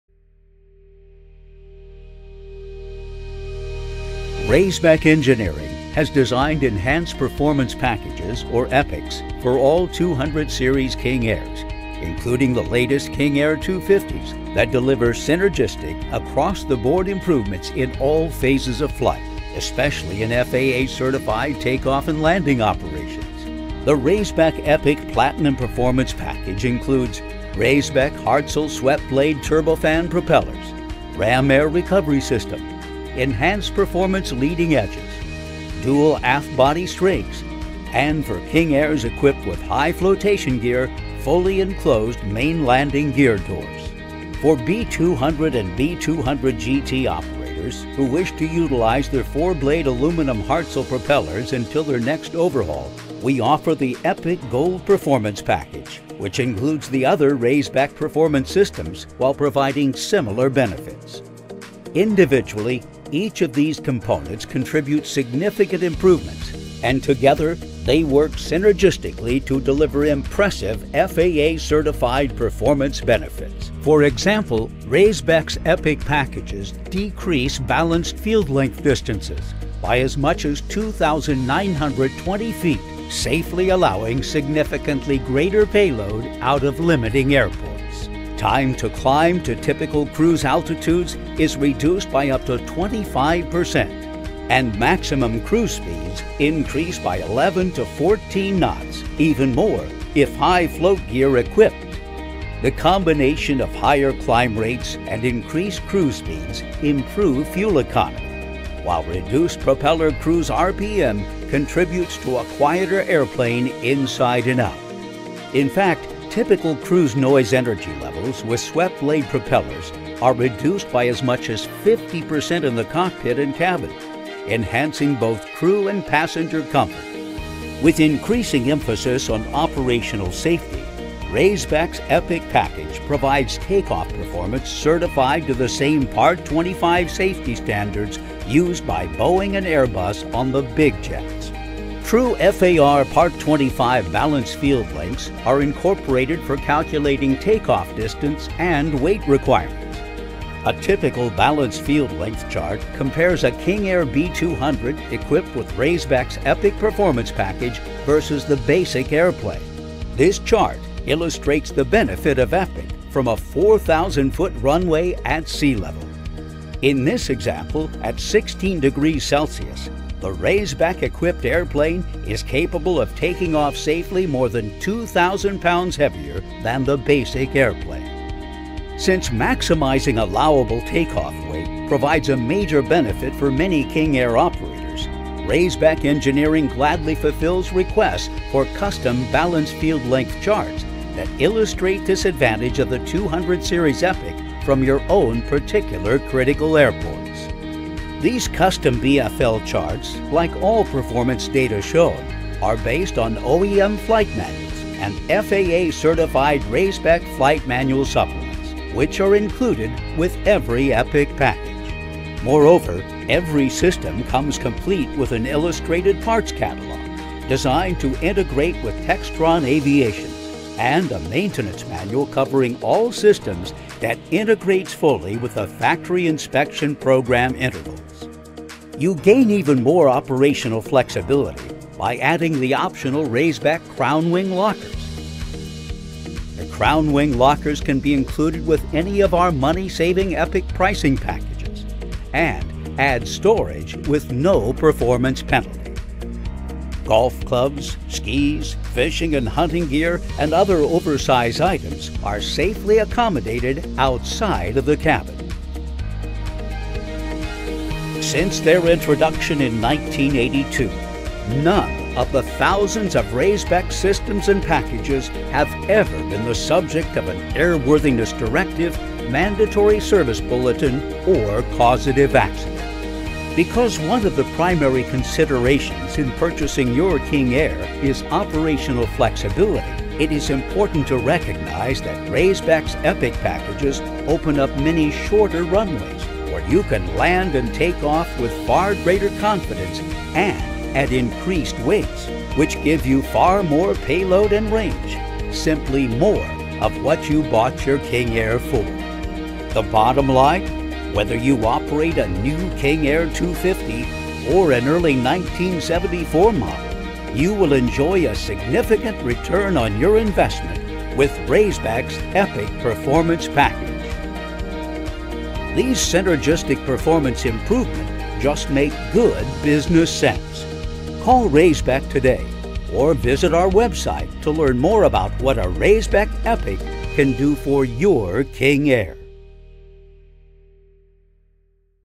Raisbeck EPIC Performance Packages for 200-Series King Airs
Raisbeck Engineering has designed Enhanced Performance Packages ("EPIC") for all 200-Series King Airs, including the latest King Air 250s, that deliver synergistic, across-the-board improvements in all phases of flight—especially FAA-certified takeoff and landing operations.